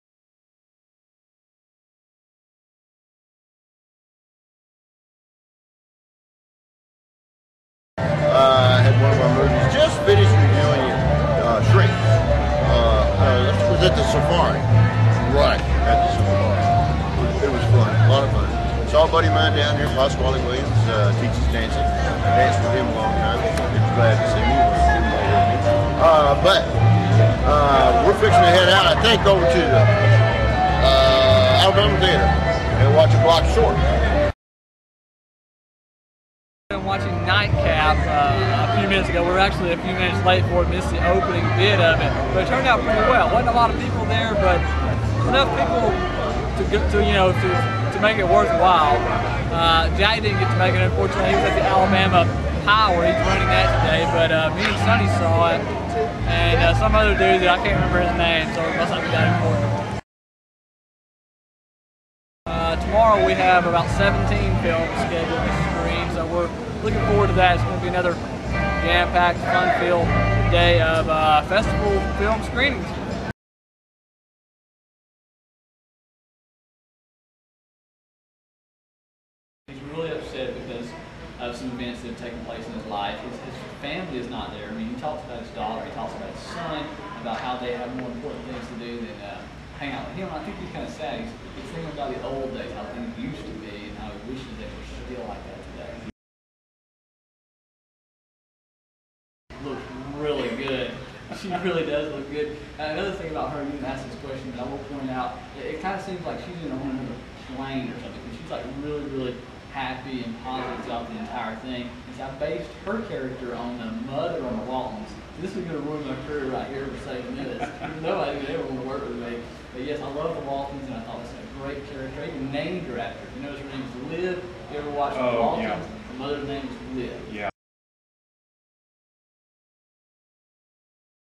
Our trip to the Sidewalk Film Festival (video)
This is the 3rd part of our recent trip to the sidewalk film festival in birmingham alabama.